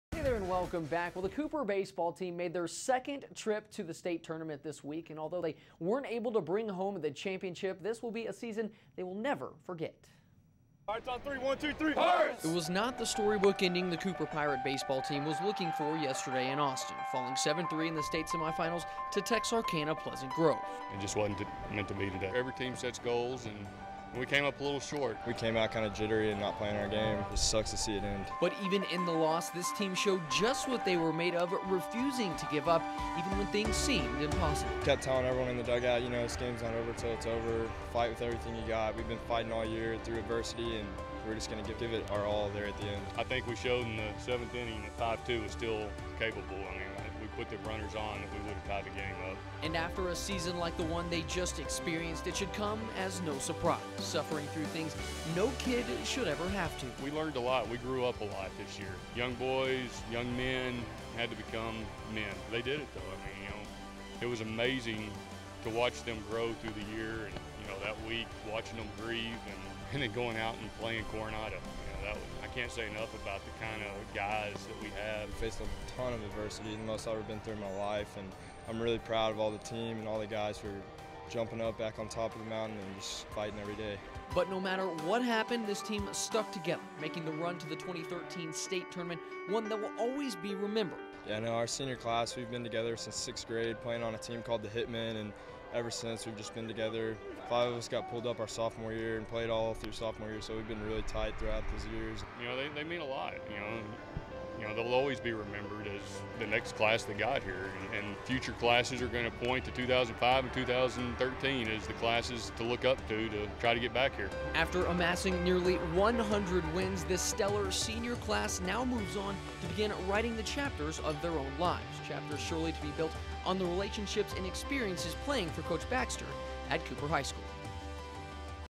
A Season to Remember: Cooper Baseball
A Package I did on the Lubbock Cooper Baseball team after their season ended at the State Tournament.